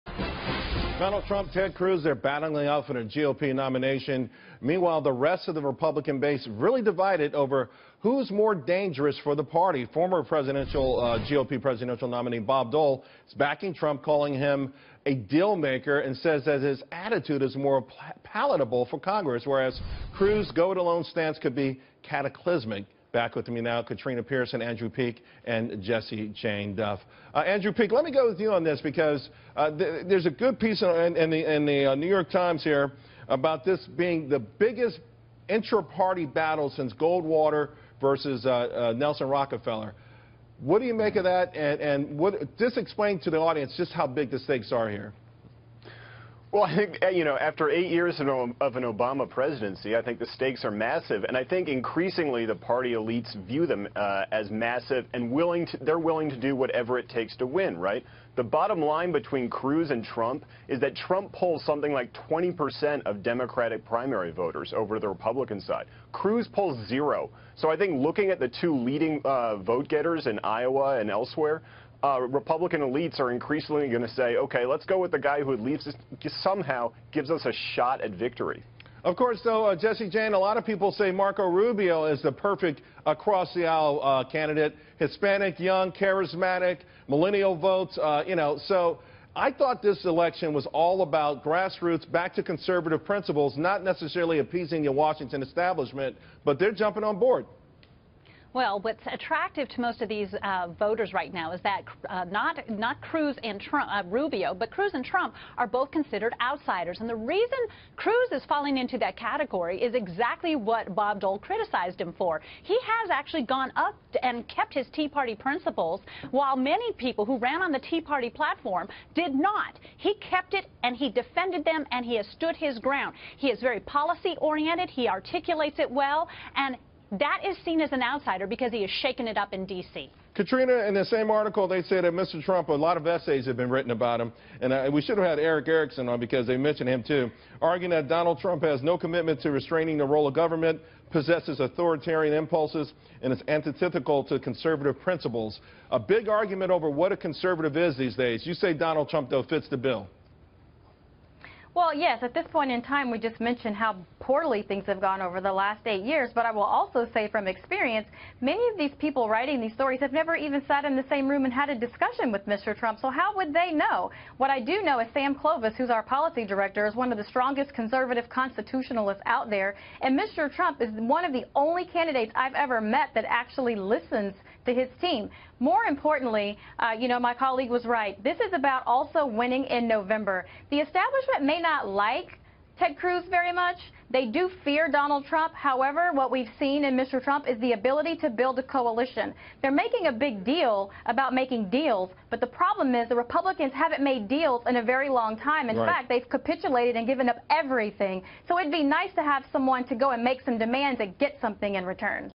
Trump, Cruz and the battle for the White House
Former U.S. Army Intelligence Officer Andrew Peek, London Policy Research Senior Fellow Jessie Jane Duff and Trump National Spokeswoman Katrina Pierson on the 2016 presidential race.